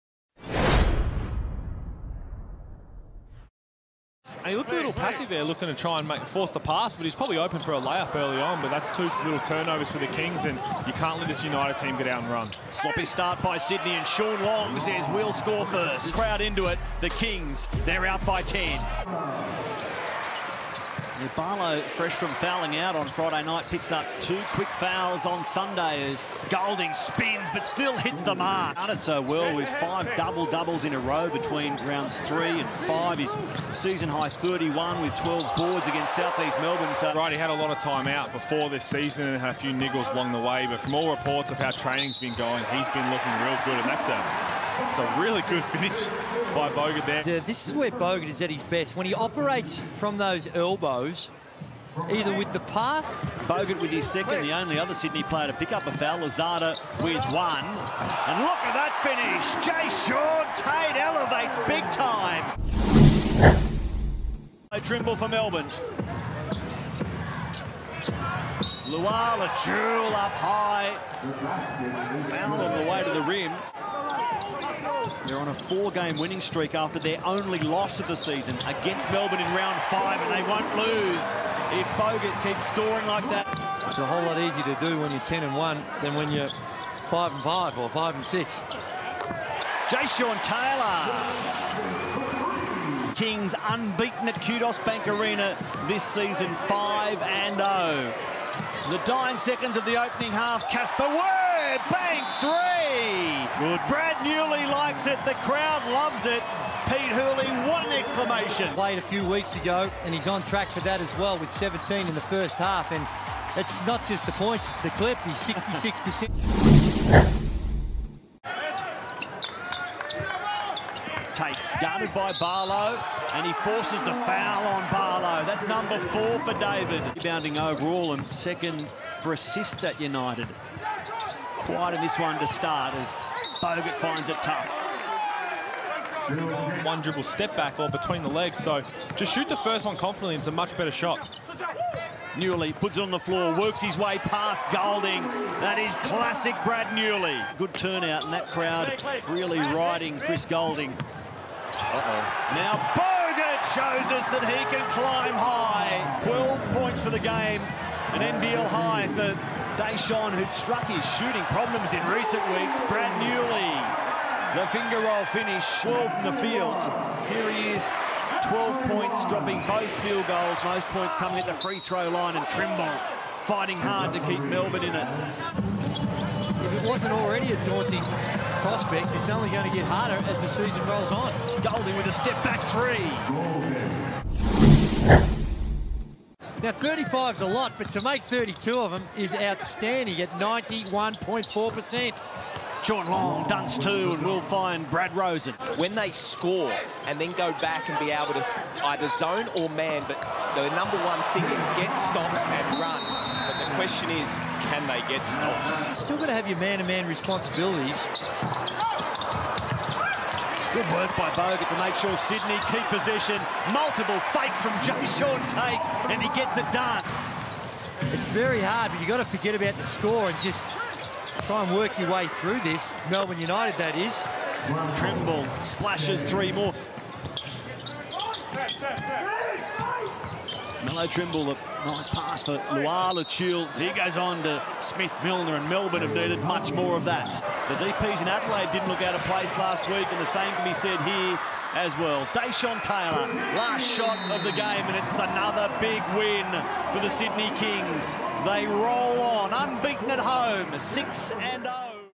Sydney Kings vs. Melbourne United - Game Highlights
Watch the Game Highlights from Sydney Kings vs. Melbourne United, 12/01/2019